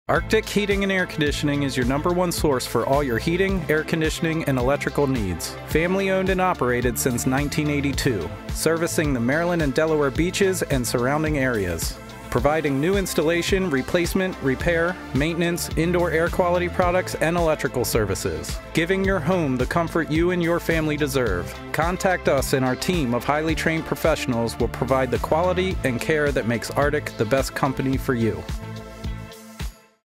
Arctic Heating & Air Conditioning - 2022 Promo Spot
A 30 second promotional spot produced by unscene productions for Arctic Heating & Air Conditioning located in Berlin, Maryland, an all around promo spot for Hooked On OC promoting the best Heating & Air Conditioning company serving the Maryland & Delaware Beaches, now also offering electrical services. unscene 2022.